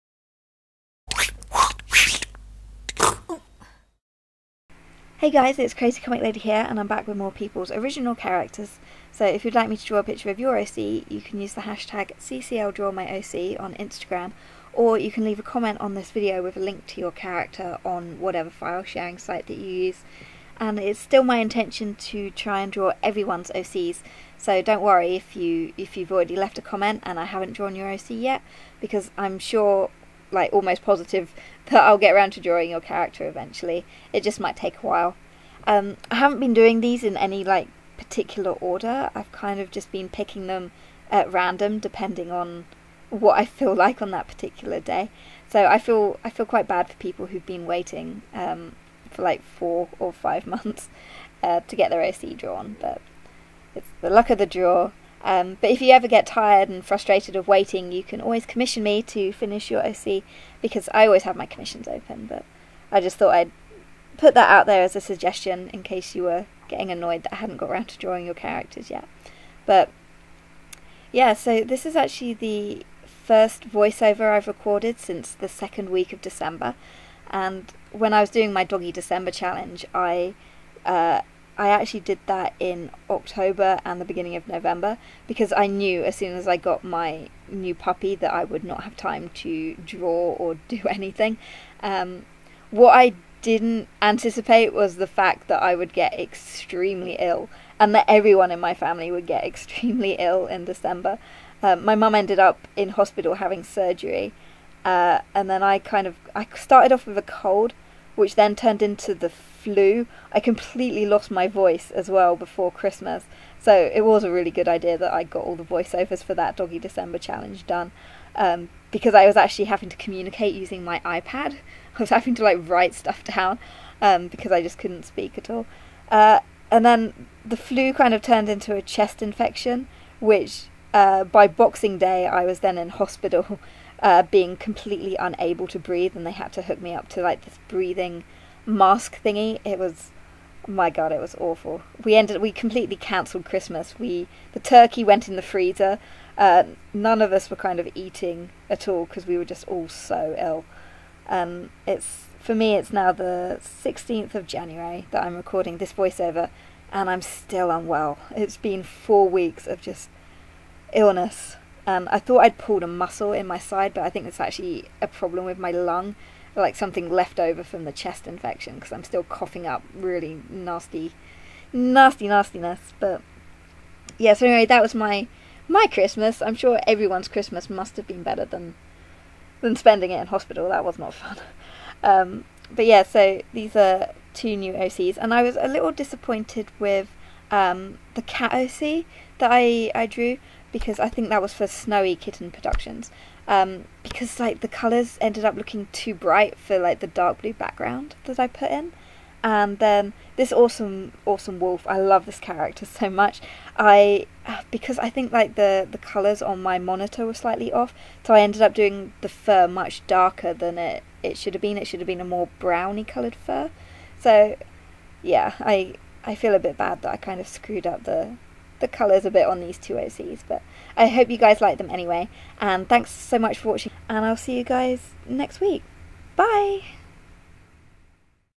*CLOSED* #10 I Draw Your OC
An OC is an original character that you've created. if you have an OC you'd like me to draw  there are 2 ways to let me know
1: use the hashtag CCLdrawmyOC on instagram or
2: comment on this video with a description and link to your character
- only one OC per person. If your OC gets drawn please don't comment with another, that's just greedy ;P

- If you only provide a pencil sketch as a reference your OC will be coloured in grey scale or may just be sketched.

-  No Guarantee that all OCs will be fully coloured.

- Please don't feel bad if your character isn't drawn, i'm only one person and I get bored ^_^

- Please be patient and don't leave rude comments. These ATCs are FREE and drawn in my spare time. If you'd like your character drawn and don't want to wait you can always commission me ^_^

Materials used:
- Canson Bristol
- Letraset and Copic Markers
- White uni ball signo gelly roll/ Sakura gelly roll
- Stickles glitter glue

I do not accept Requests.
Trades are closed.

Commissions are available!